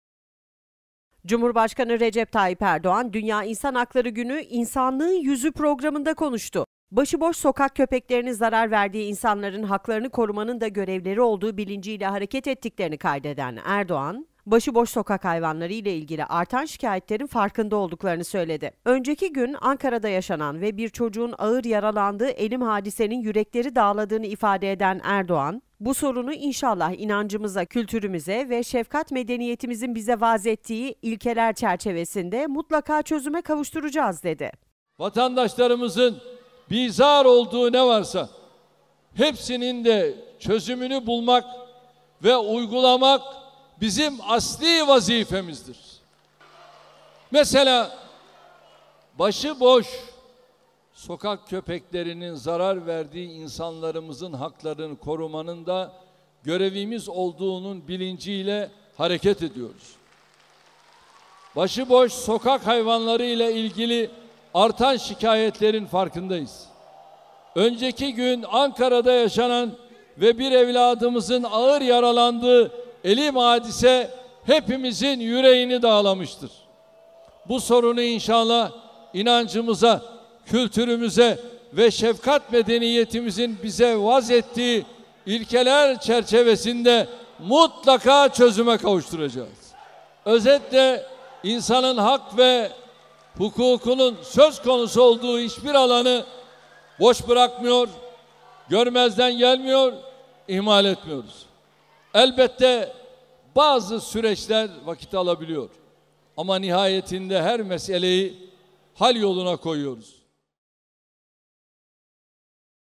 Cumhurbaşkanı Erdoğan Başıboş Köpek Sorunuyla İlgili Net Konuştu
Cumhurbaşkanı Recep Tayyip Erdoğan, "Dünya İnsan Hakları Günü İnsanlığın Yüzü Programı"nda konuştu.

Başıboş sokak köpeklerinin zarar verdiği insanların haklarını korumanın da görevleri olduğu bilinciyle hareket ettiklerini kaydeden Erdoğan, başıboş sokak hayvanlarıyla ilgili artan şikayetlerin farkında olduklarını söyledi.

Önceki gün Ankara'da yaşanan ve bir çocuğun ağır yaralandığı elim hadisenin yürekleri dağladığını ifade eden Erdoğan, "Bu sorunu inşallah inancımıza, kültürümüze ve şefkat medeniyetimizin bize vaz ettiği ilkeler çerçevesinde mutlaka çözüme kavuşturacağız." dedi…